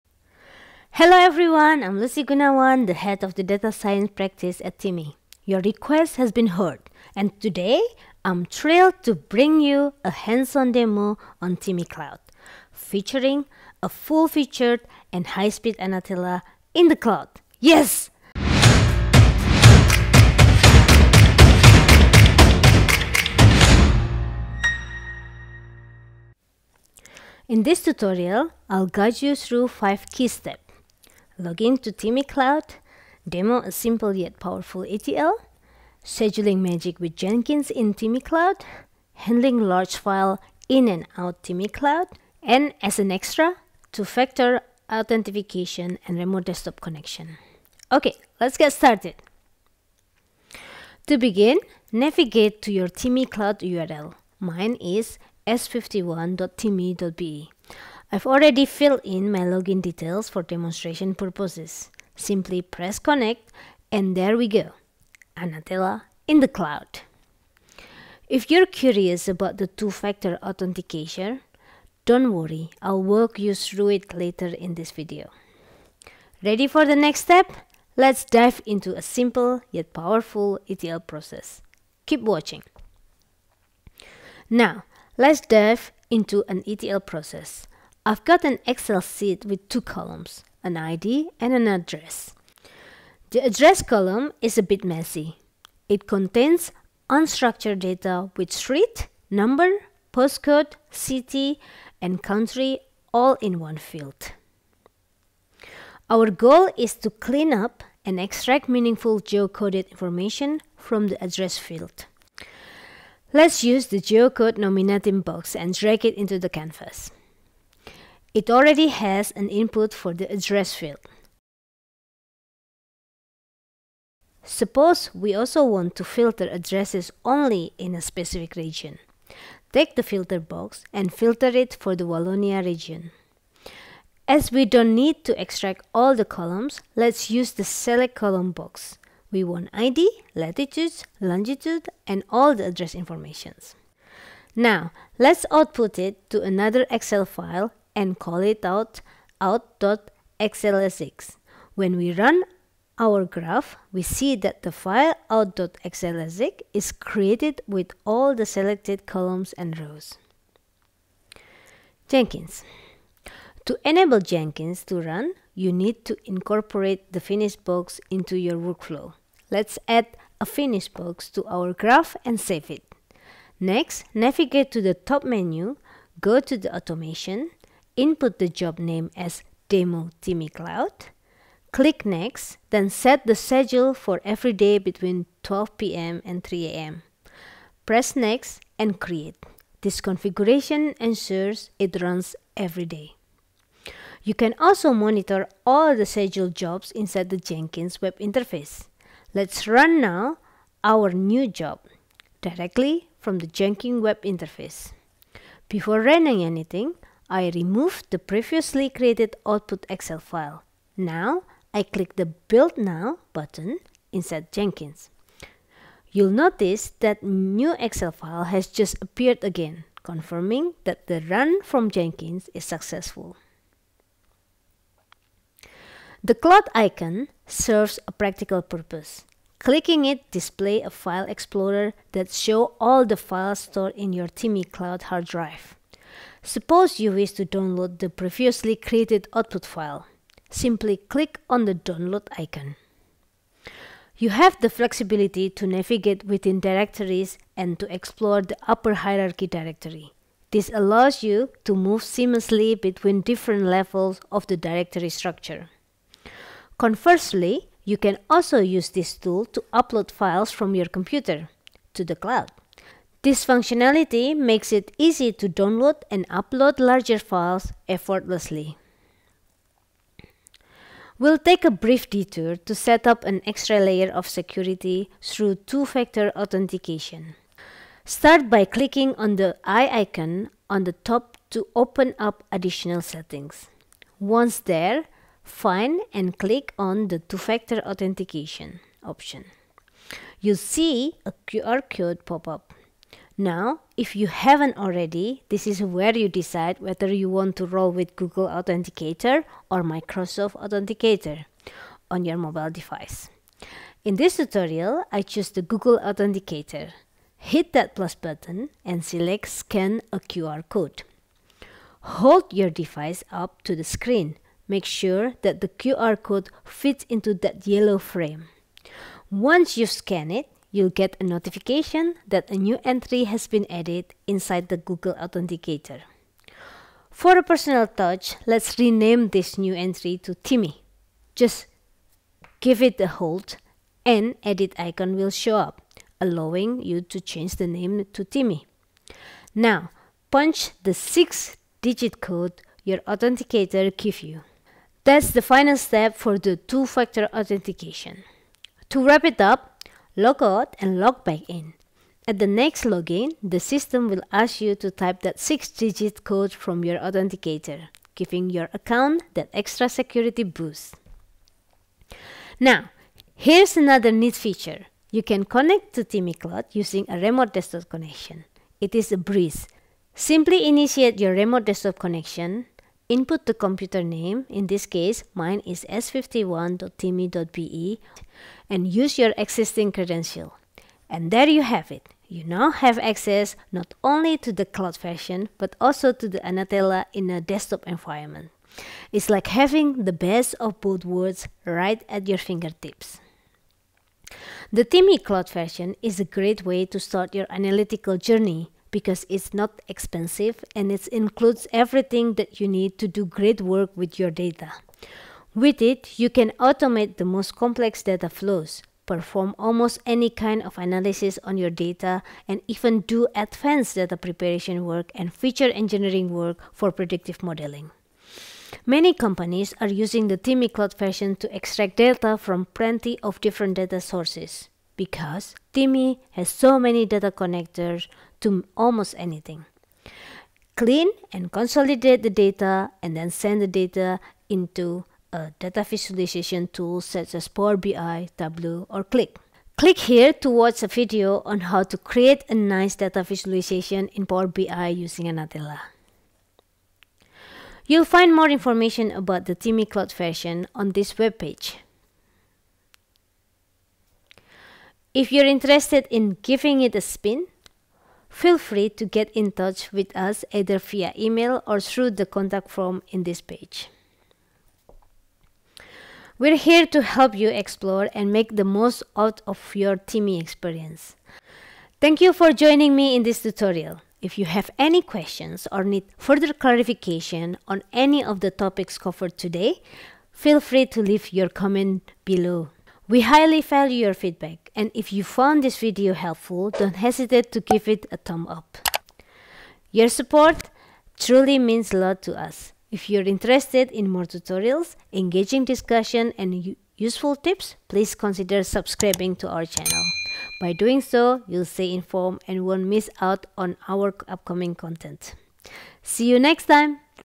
TIMi in the cloud: The demo !
A hands-on demo of TIMi Cloud, featuring a full-featured
and high-speed Anatella in the cloud! Yes!

00:00 Intro
00:26 Agenda
00:49 Log-in into TIMi cloud
01:23 Demo of a Powerful ETL: Unlimited Cleaning and Geocoding of postal addresses
02:41 Scheduling Magic with Jenkins
03:45 Transferring Large Files
04:31 Extra security: 2-factor authentication
06:01 Remote desktop connection (RDP)
06:34 TIMi Cloud: For what and for who?
07:51 Outro